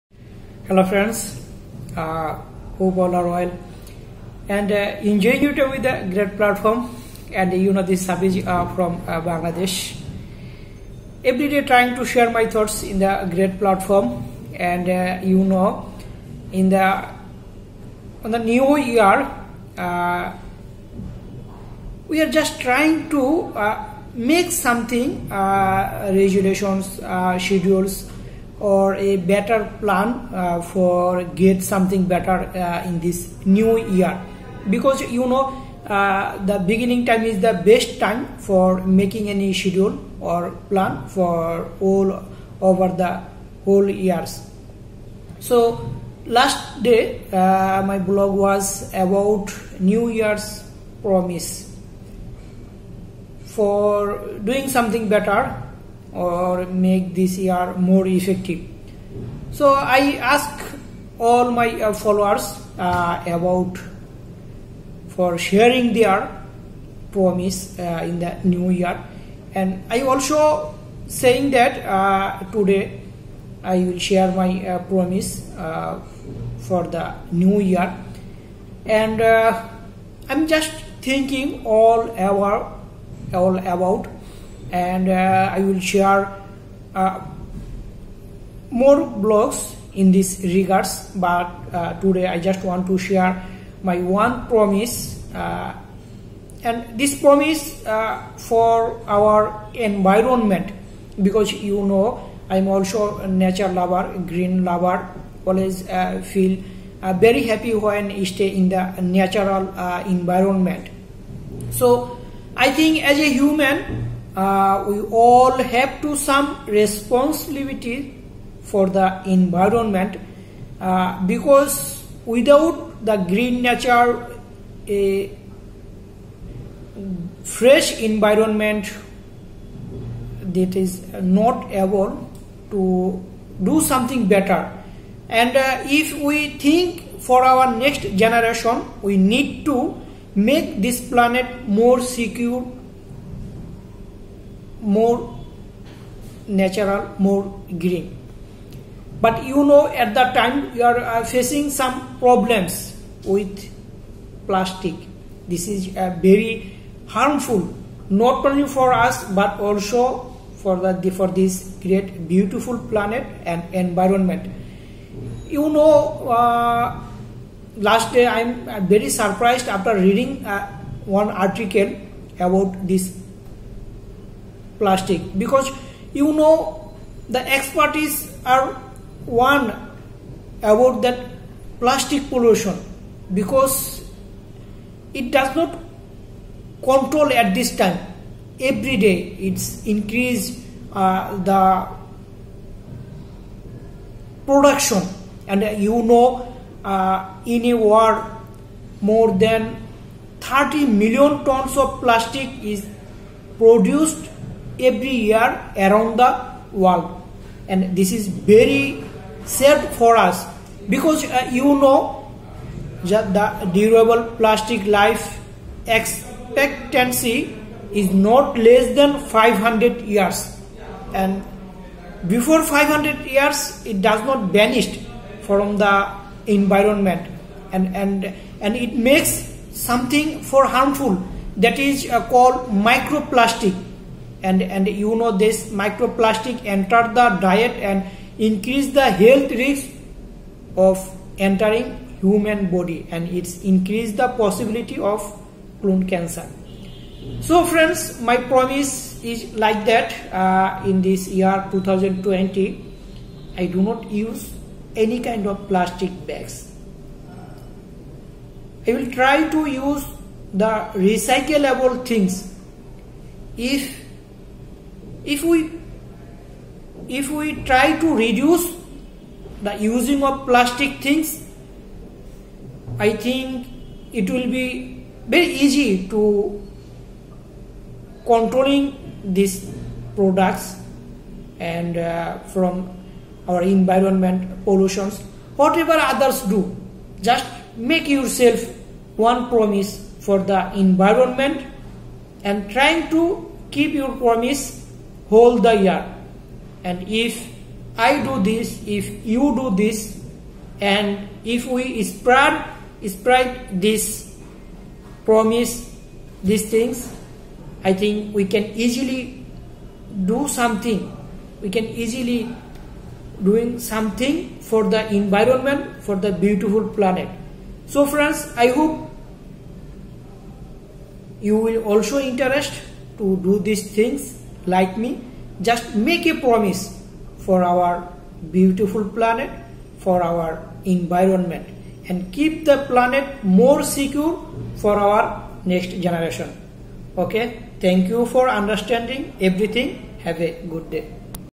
We all have a Responsibility to the Environment || Know my New Year’s Oath
We all have a responsibility to the environment, because without the right environment it is impossible for us to survive. So in order to survive well, the environment must be kept pollution-free, and we should all work together in this regard.